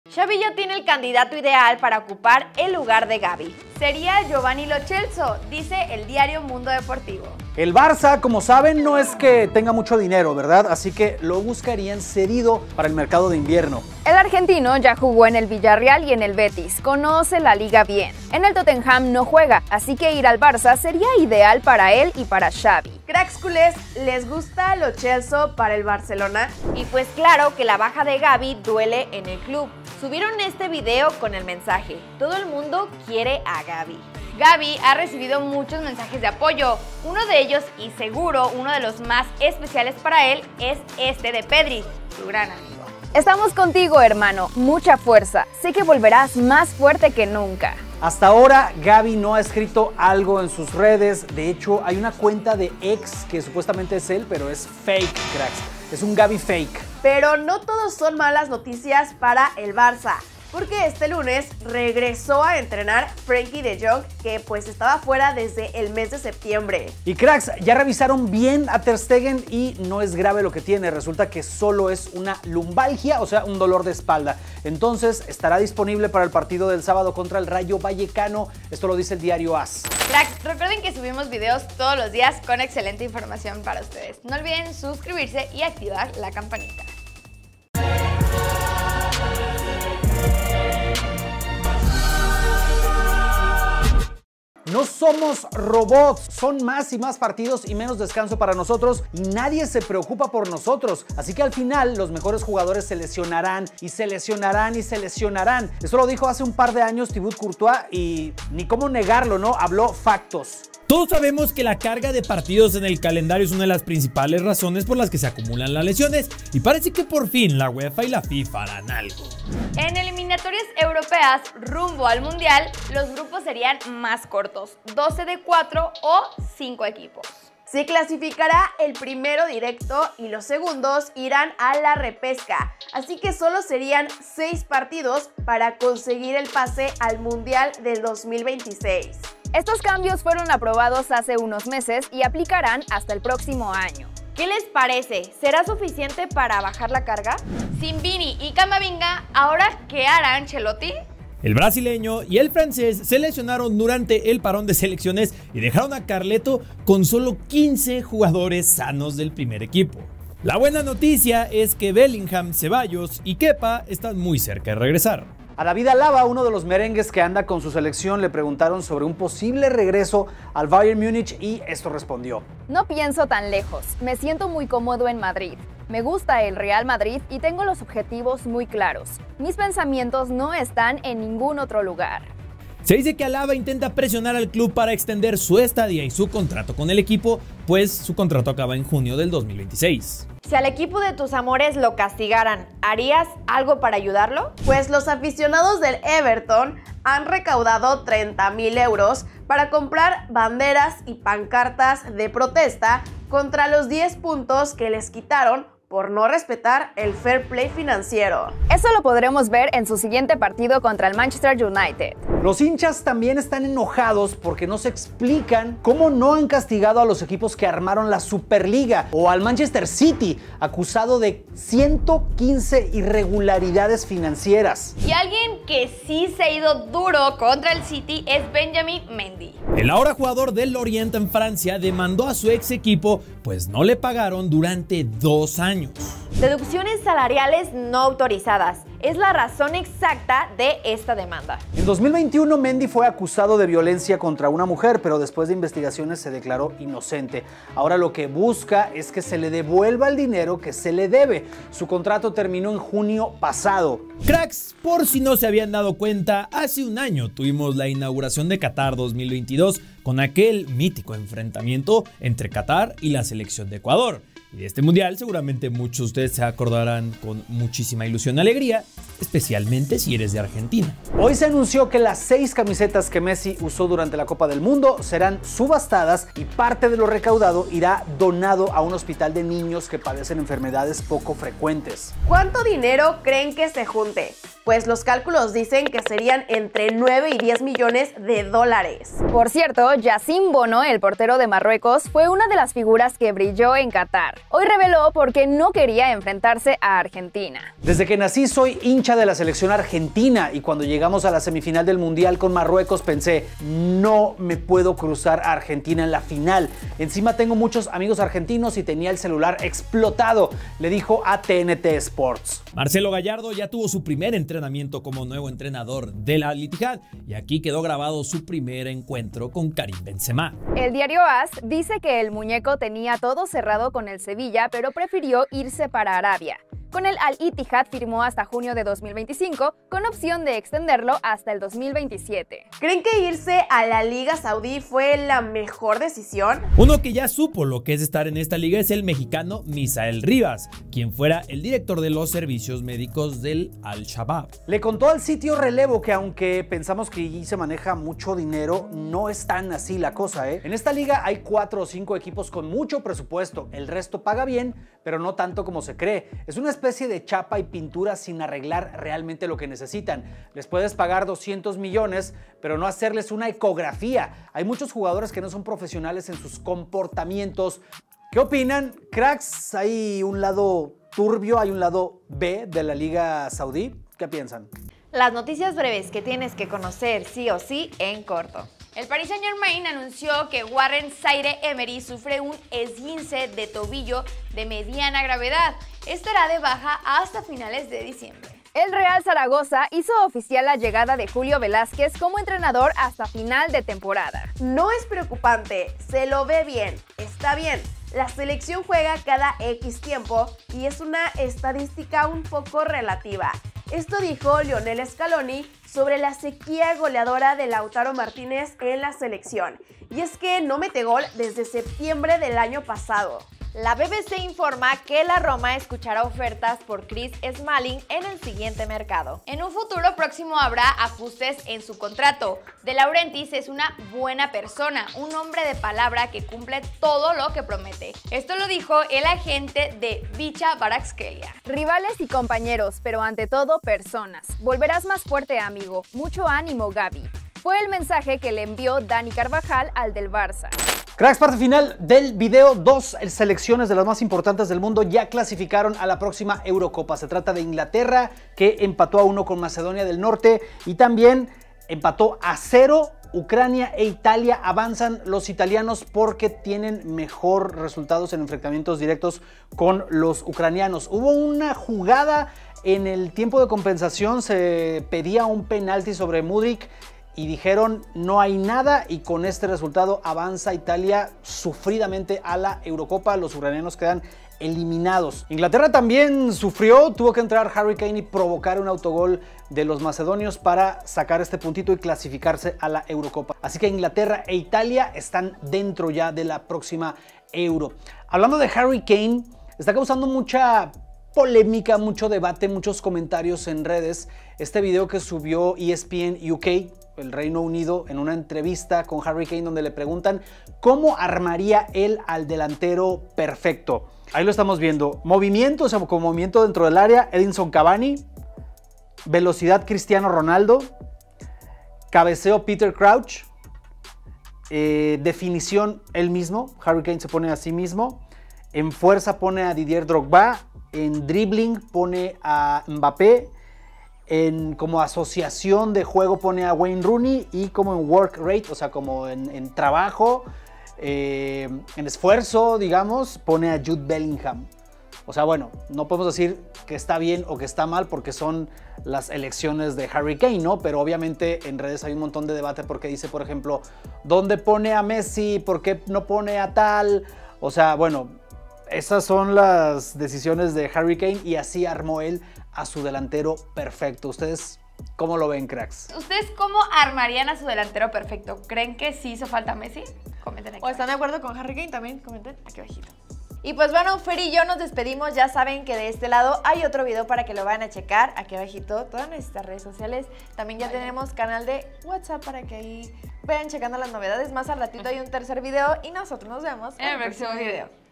Llegaría el REEMPLAZO de GAVI | Subastarán ‘TESOROS’ de MESSI | EVERTON PROTESTA | Critican a KANE
El candidato ideal para reemplazar a Gavi. Buscarían cedido a Lo Celso. Mensajes de apoyo a Gavi. Frenkie de Jong regresó a entrenar al parejo, Ter Stegen estará disponible para jugar ante el Rayo. Quejas por la carga de partidos que provocan lesiones. Cambios en las eliminatorias europeas con menos partidos rumbo al mundial 2026. Bellingham, Ceballos y Kepa más cerca de regresar al primer equipo del Real Madrid. Everton quiere que castiguen al Manchester City. Benjamin Mendy demanda al City. Subastarán 6 camisetas que usó Messi en el Mundial. Yassine Bono reveló por qué no quería enfrentarse a Argentina. Críticas a la Liga Saudí. Polémica por elección del delantero perfecto de Harry Kane.